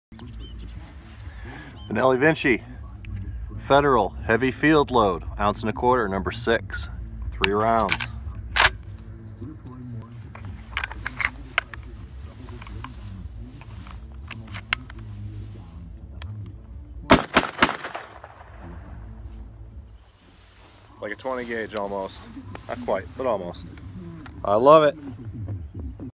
How does the Vinci handle recoil? Like a pump 20ga...? Not quite, but close
People often ask how much recoil a certain shotgun has. With auto loaders, I find this a mute point, but the inquirers persist. So, here is a demonstration with Federal heavy field loads (1-1/4oz. @ 1220fps). These loads are fantastic on game and I have killed a number of rabbits and doves with the #5 and #6 loads. Recoil is similar to a 3" 20ga shell out of a pump or break action, but not near as soft as an auto loading 20ga and definitely not a 20ga trap load. What matters more with the Vinci is its ability to stay on target for faster follow up shots.
-Drake